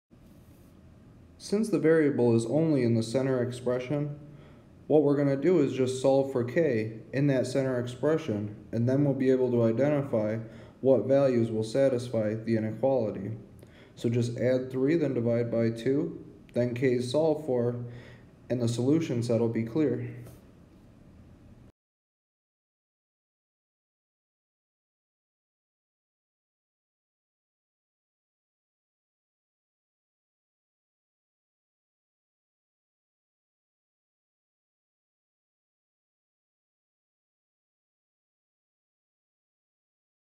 Compound Inequality: Solve and graph -6 ≤ 2k-3 ≤ 17
Tony from SVSU Micro Math helps you solve a compound inequality, then graph it and write the solution using interval notation.  A “compound inequality” is two or more inequalities connected by “and” or “or”.  In this problem, the compound inequality is written as a “double inequality”.

Problem: Solve and graph -6 ≤ 2k-3 ≤ 17